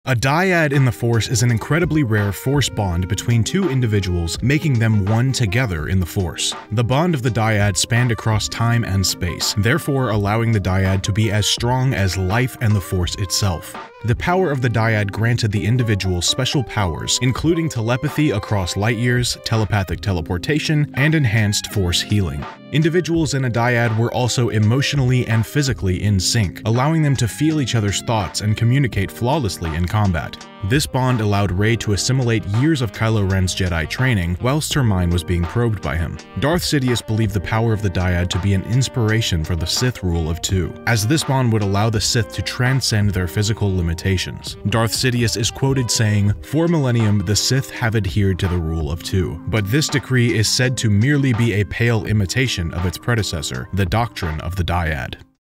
Force Dyad Explained | No BS Lore
Tired of long-winded, unnecessary lore videos? I got you covered. This is a series where I answer STAR WARS lore questions in the quickest, most entertaining way possible. In this video, we define everything there is to know about a Force Dyad.